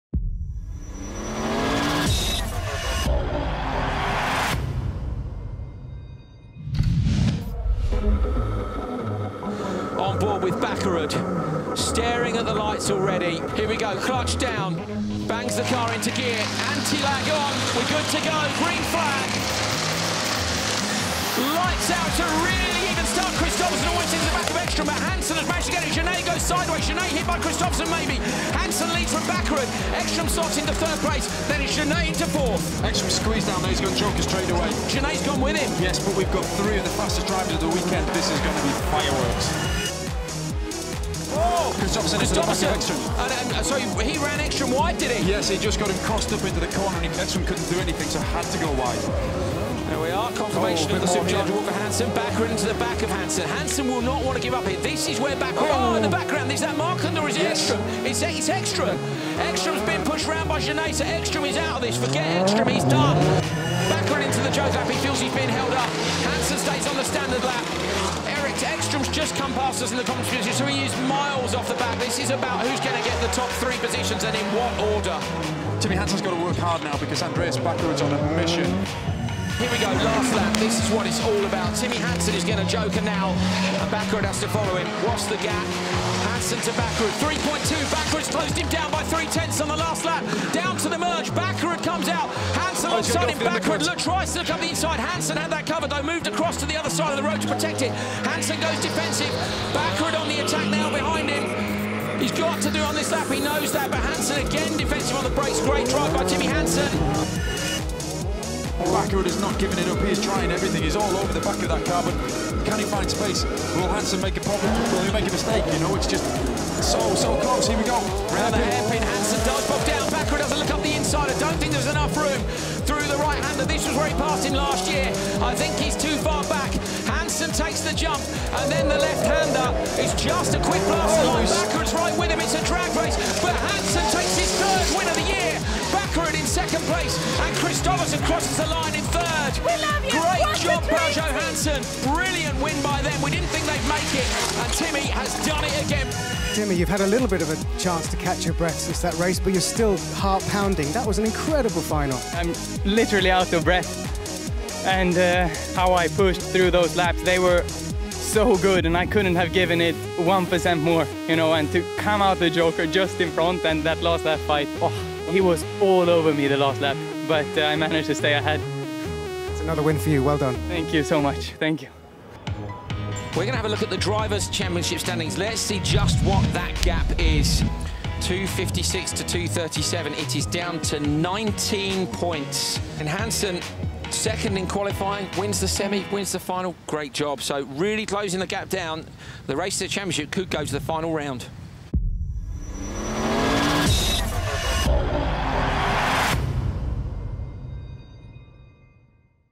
2015 Turkey RX Final Highlights - World RX Rallycross
The best of the action from the Turkey RX Supercar final featuring Anton Marklund, Johan Kristoffersson, Timmy Hansen, Andreas Bakkerud, Mattias Ekstrom and Davy Jeanney at Istanbul Park, Tuzla, Turkey